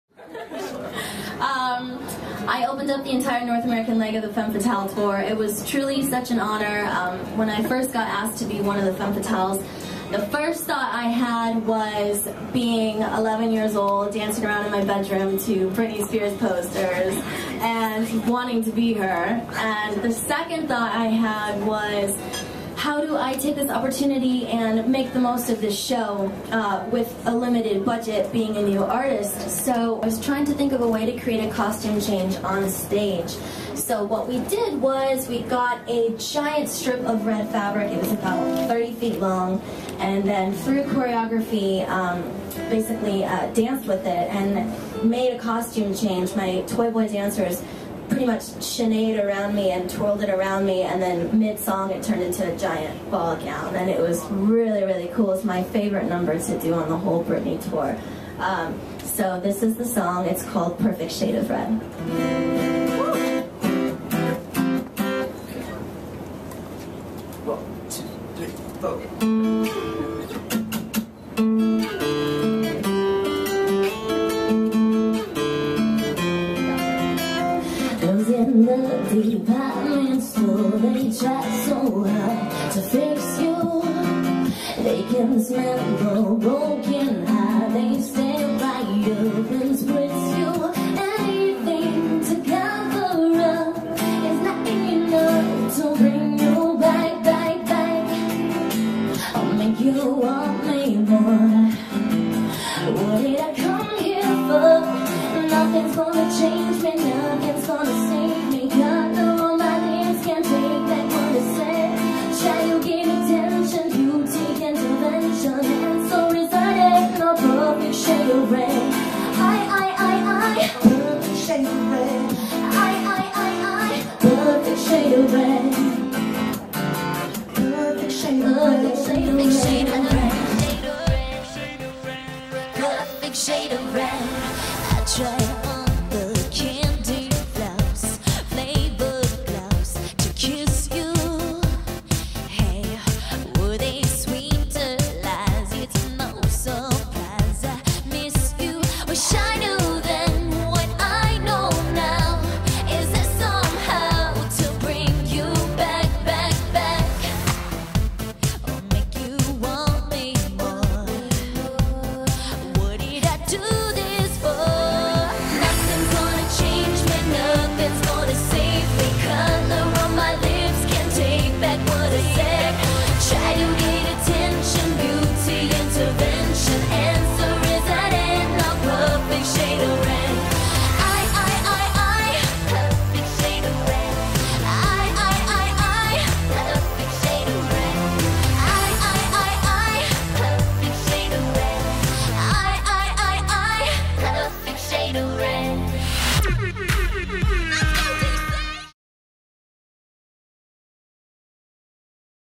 Perfect Shade of Red: Acoustic vs Tour
Jessie performs an acoustic set in the AMEX SkyBox for 2011 New York Fashion Week and talks a little bit about her creative process for the Femme Fatale Tour.